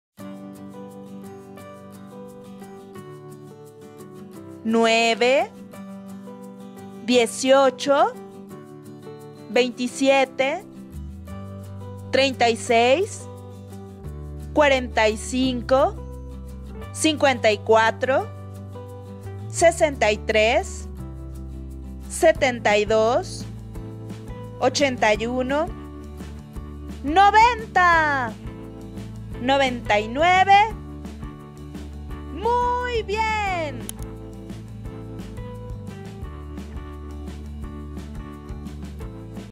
9 en 9 hasta el 99 - Videos Aprende
Este video de 9 en 9 hasta el 99 es un video sencillo para seguir aprendiendo a contar corazones.